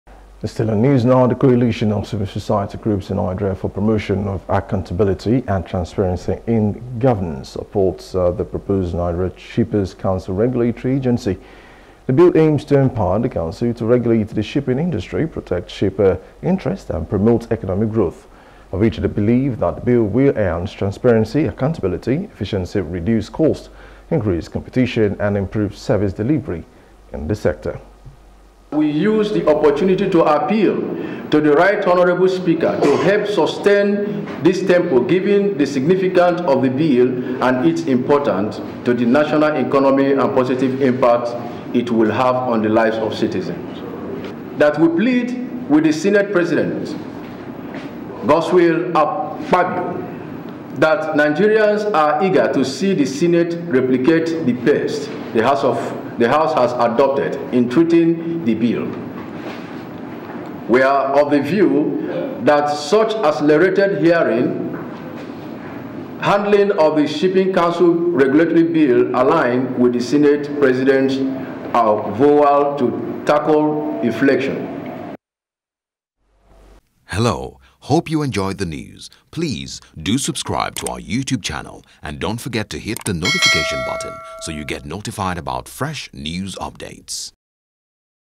Csos Back Shippers Council Regulatory Bill
The Coalition of Civil Society Groups in Nigeria for Promotion of Accountability and Transparency in Governance (CCSGNPATG) supports the proposed Nigeria Shippers Council regulatory agency.